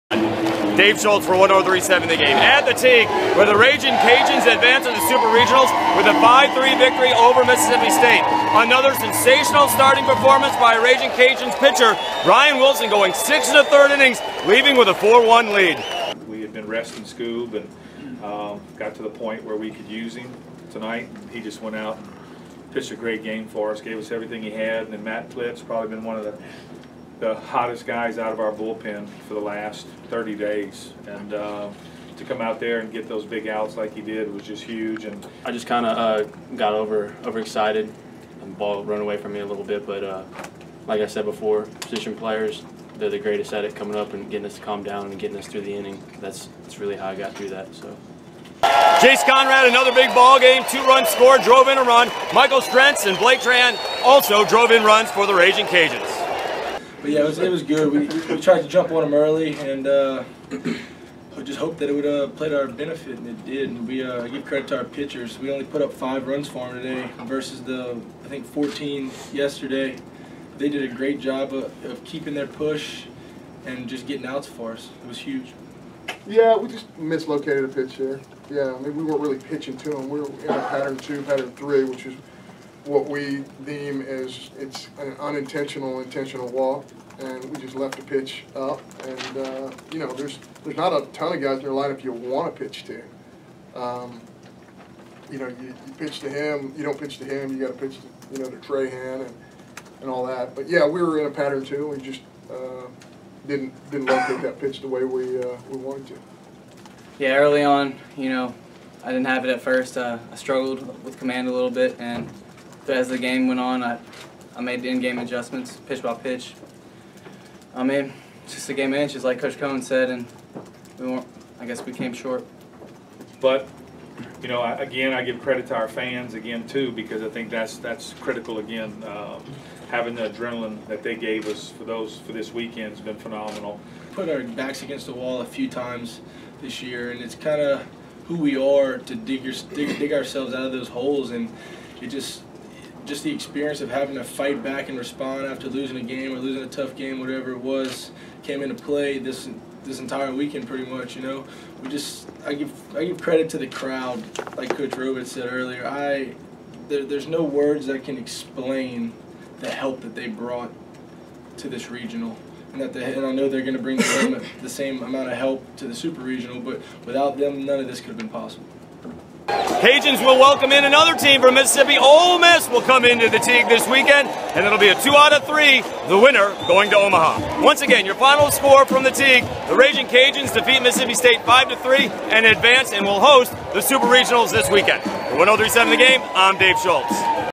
Cajuns Advance to the Super Regional
University of Louisiana Ragin' Cajuns beat Miss St 5-3 and will host Ole Miss in the Super Regional